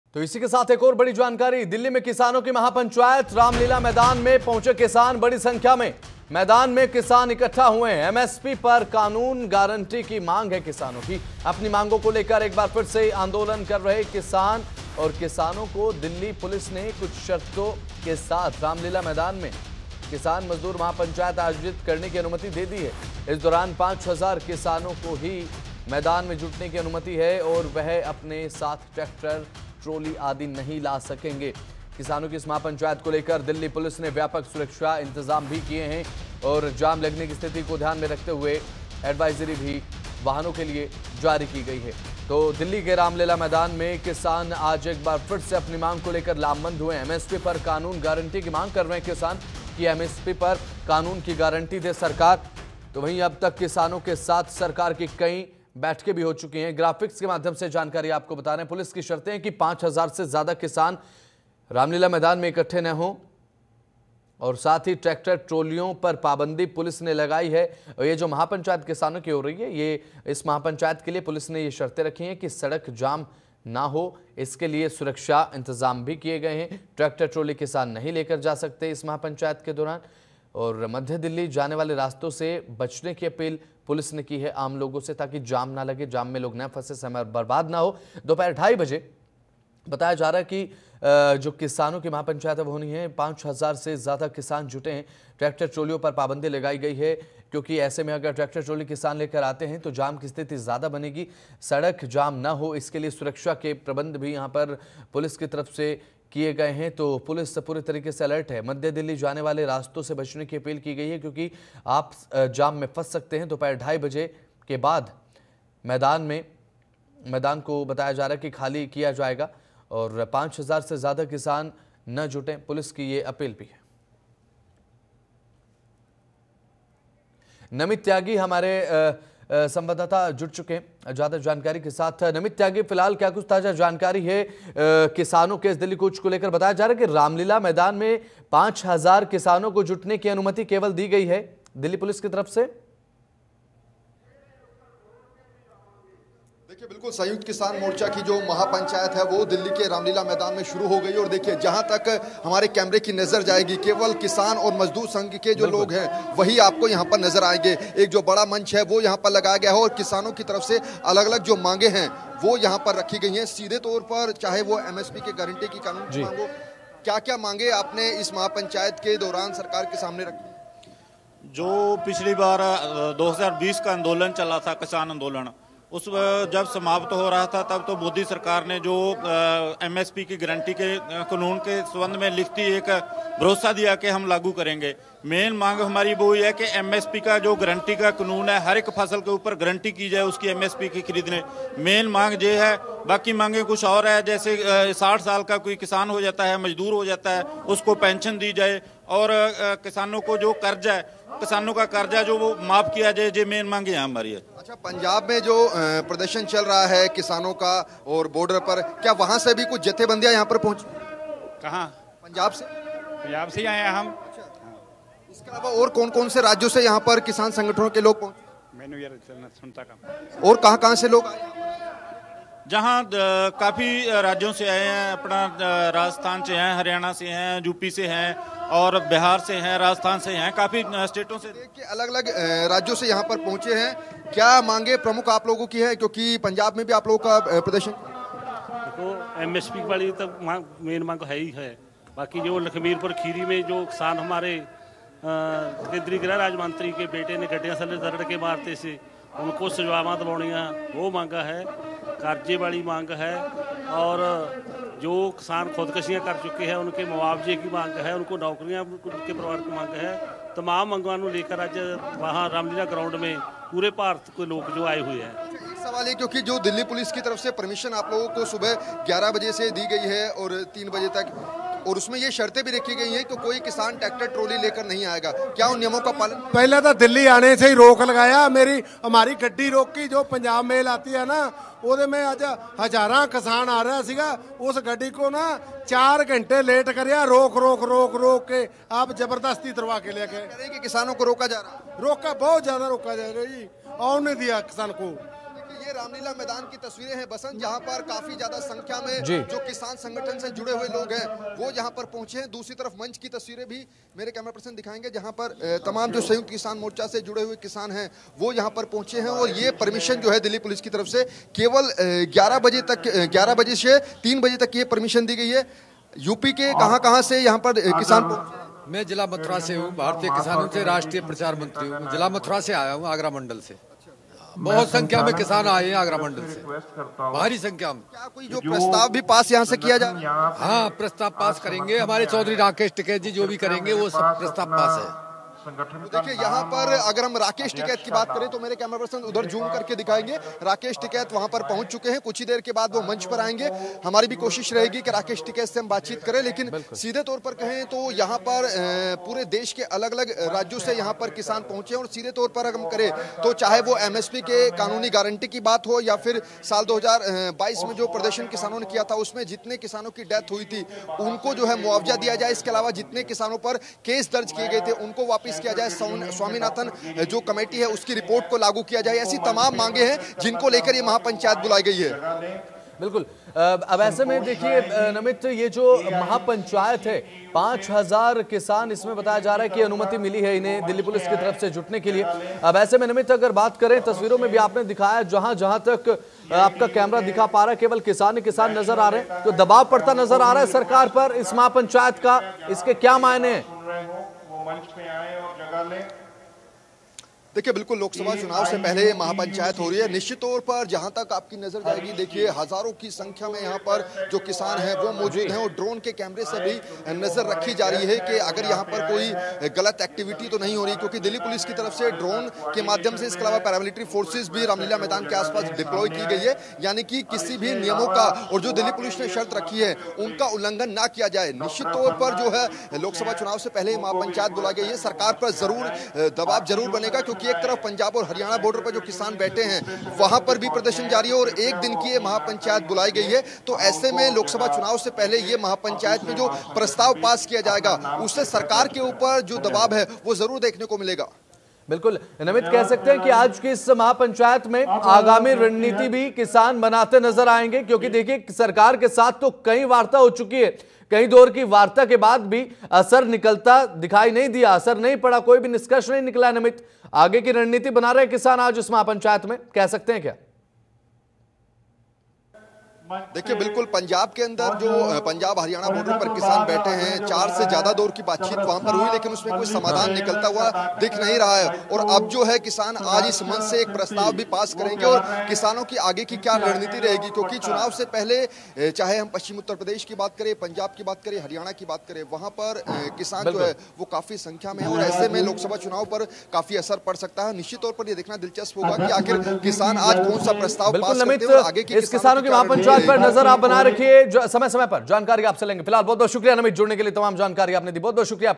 Kisan Mahapanchayat: Delhi में किसानों का हल्ला बोल! बड़ी संख्या में Ramleela Maidan पहुंचे किसान
Kisan Mahapanchayat: Delhi में किसानों का हल्ला बोल! बड़ी संख्या में Ramleela Maidan पहुंचे किसान | Breaking News